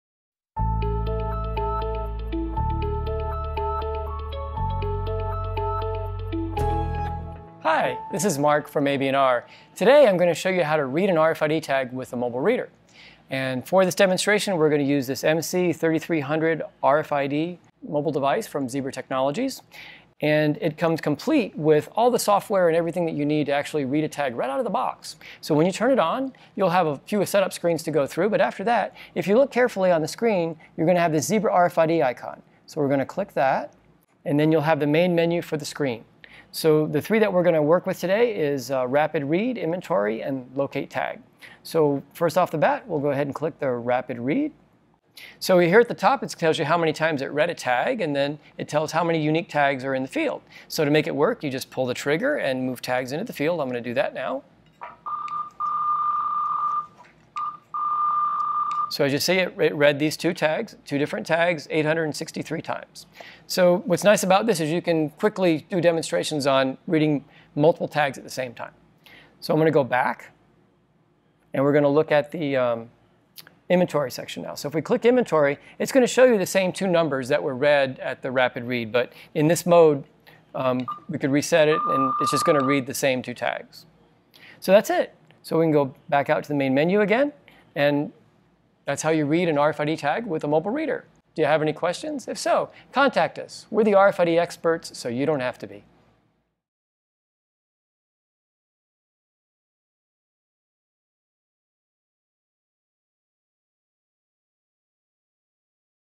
How To Read A Tag With A Mobile RFID Reader
Follow along step-by-step while we use a Zebra MC3300R MobileReader to read an RFIDtag.